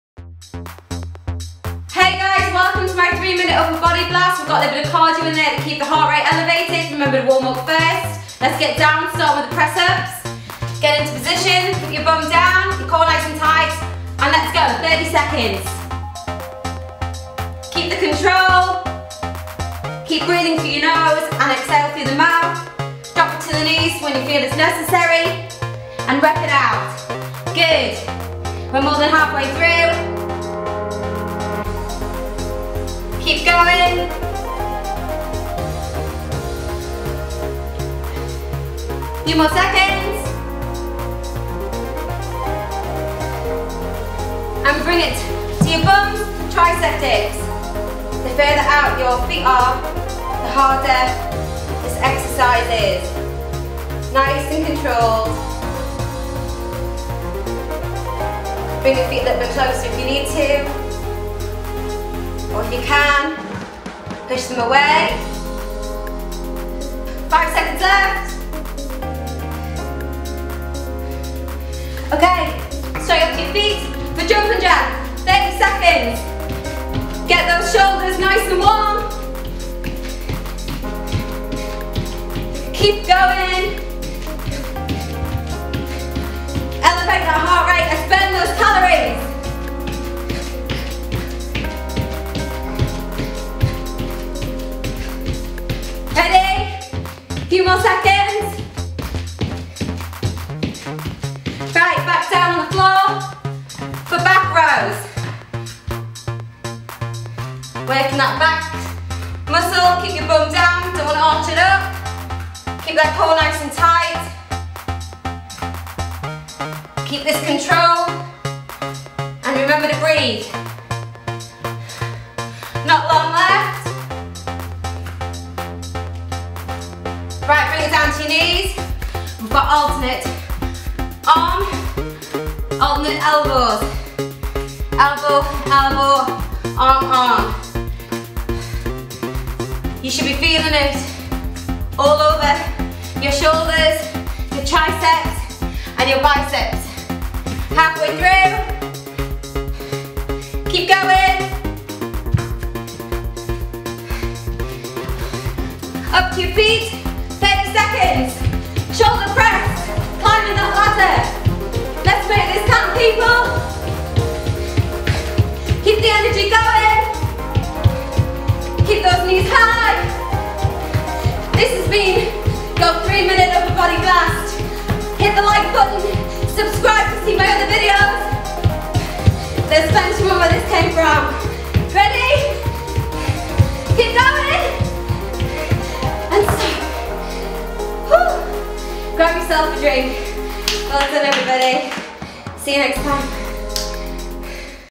Amazing 3 Minute Upper Body Workout
3 Min Cardio Blast - MORE INFO BELOW :)

Press ups, tricep dips, jumping jacks, back rows, arm arm elbow elbow, ladder climbing

First up its press ups!
If you want to banish bingo wings, these are GREAT for toning your arms!
If you struggle with these, try having your knees on the floor or if you have an exercise ball, rest your legs on that!

Next: Tricep Dips
These focus on your forearms, shoulders, chest and lower back, what more could you want!

Next: Jumping Jacks
Here’s a little bit of cardio for you to get the blood pumping round your arms!
If you’re getting tired, don’t raise your arms as high :)

Next: Back Rows
Bingo wings and backs on this one! Take them slowly if it’s tough, doing one for every two I do.

Next: Arm arm elbow eblow!
This one looks easy, but it’s a tricky one a few times around! Just think of all that fat you’re burning and all the tone and definition you’re gaining!

Last but not least: Ladder climbing
The last push! Work those arms and burn that fat :)